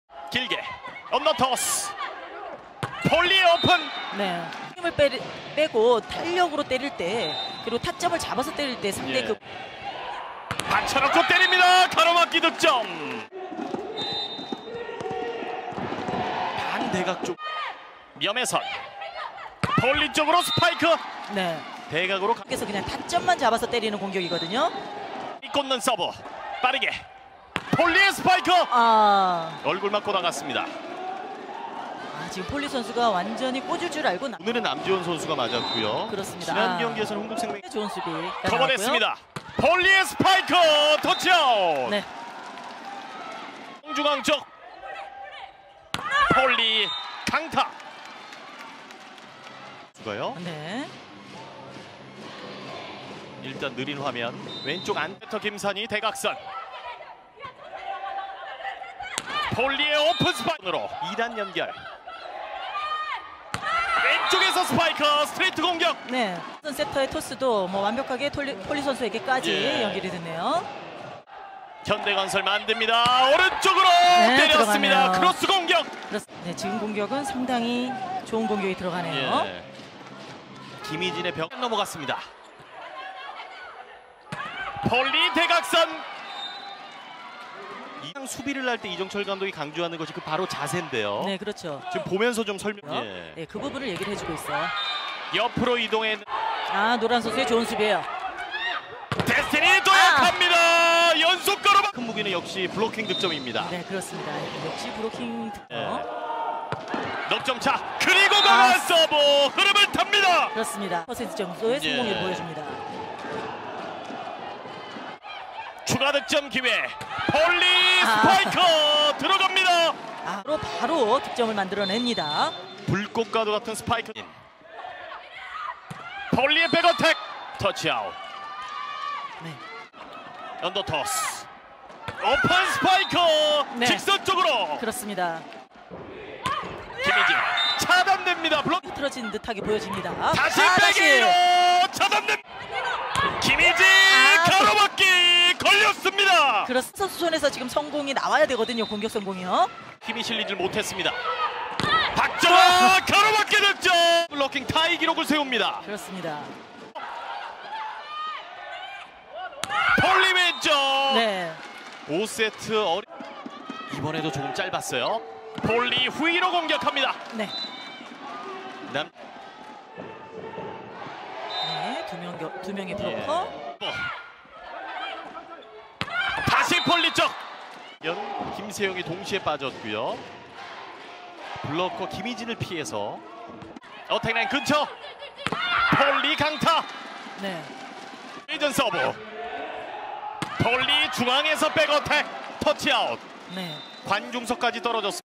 Polina Rahimova vs IBK Volleyball Highlights [kovo Round 5]
she had a strong game this match against Hooker. content to spotv